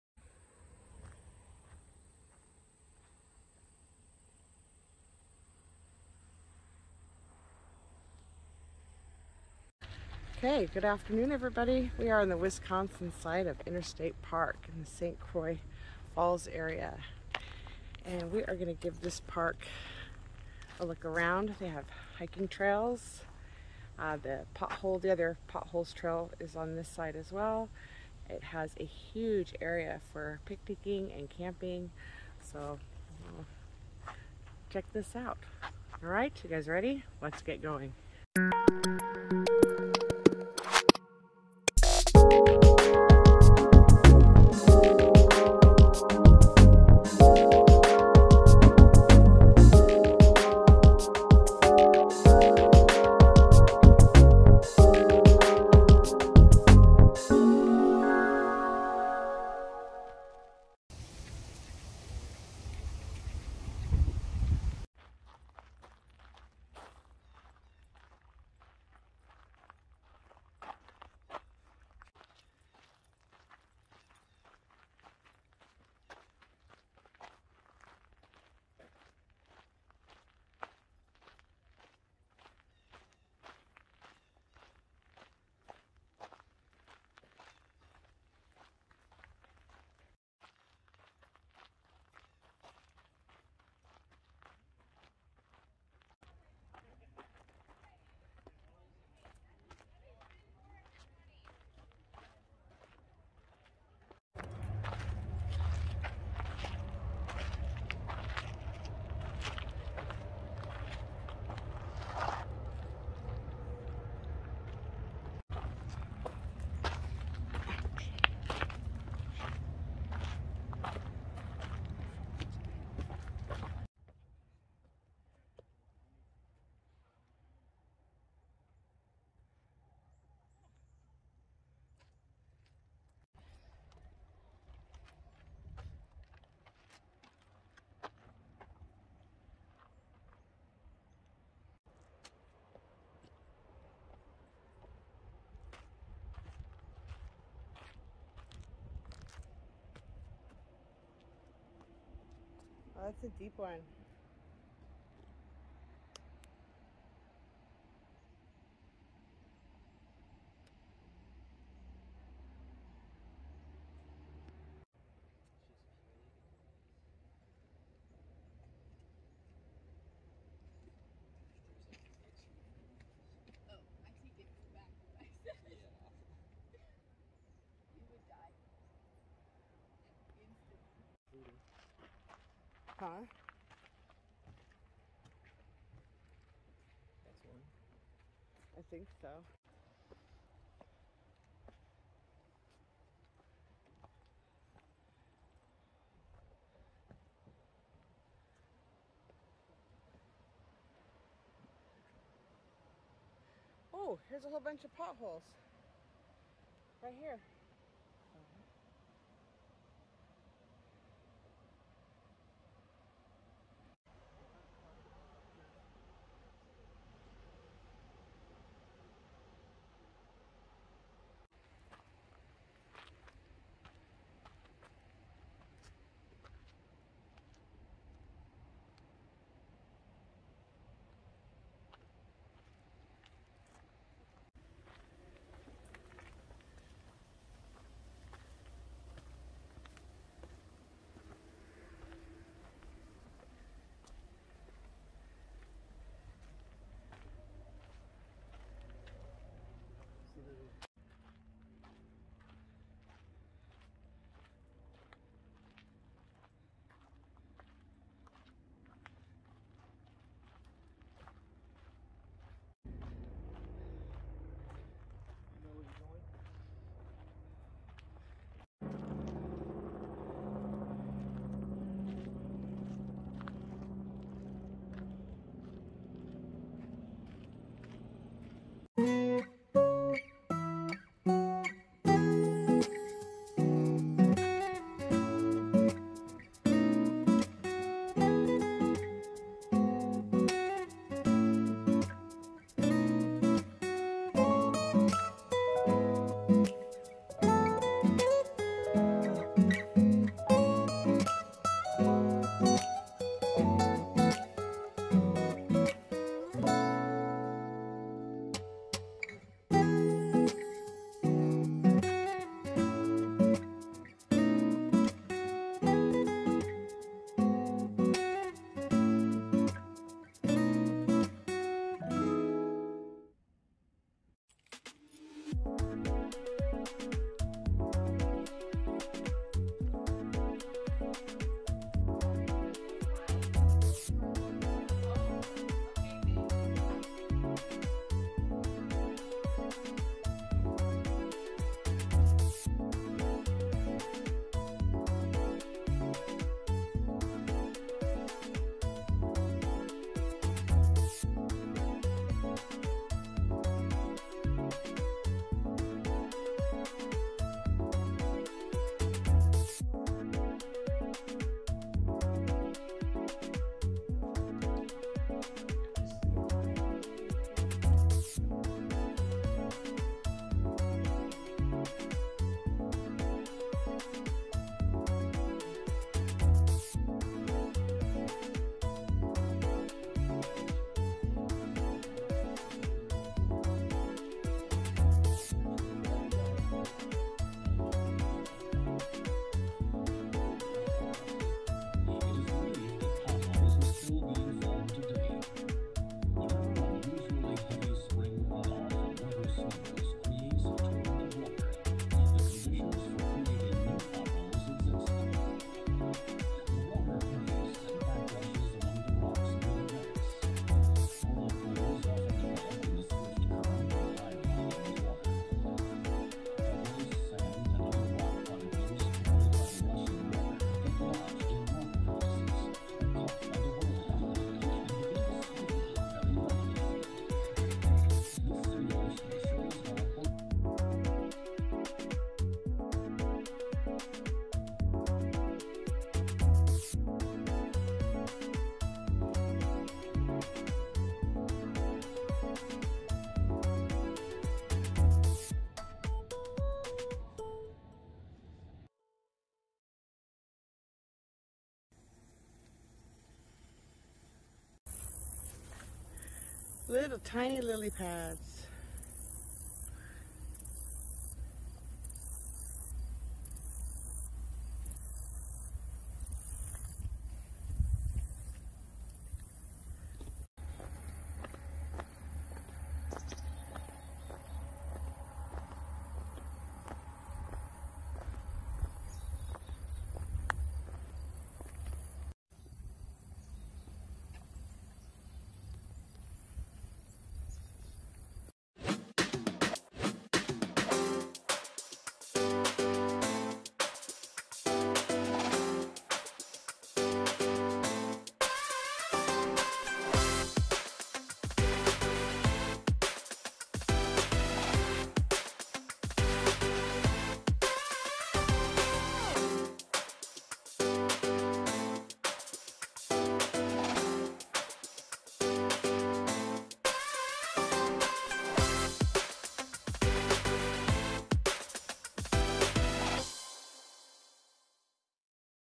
St. Croix National Riverway ~ Wisconsin ● Potholes Trail and more
St. Croix National Riverway

It is located on the Wisconsin side of the river across from Taylor Falls, Minnesota. The vistor center is in the St. Croix Falls area. The park itself is located a few miles away back across the highway.

There are campgrounds, a picnic area, a lake, and a boat launch for river access. The Ice Age Interpretive Center is located within the park, and there is a network of trails that all interconnect. We did the Potholes Trail that bus the sister trail to the trail on the Minnesota side.

This is a great area to stay in and explore while traveling through the area. It is definitely worth your time.